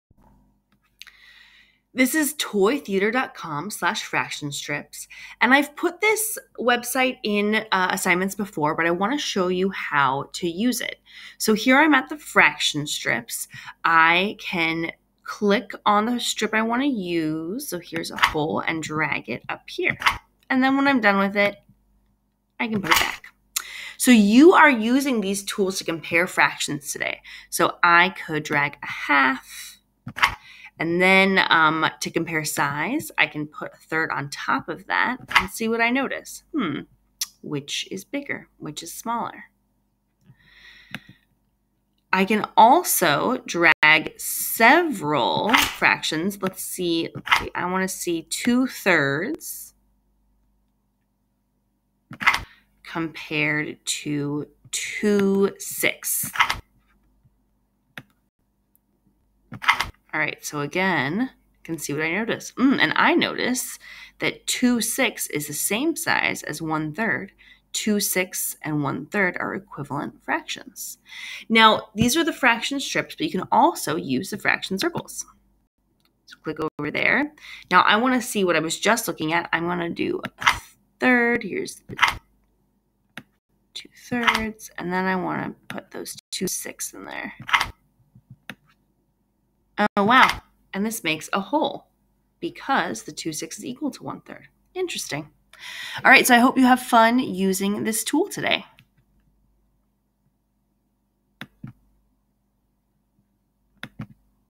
Using Fraction Strips From Toy Theater
Watch this to see how to use the Fraction Strips on Toy Theater. Enjoy!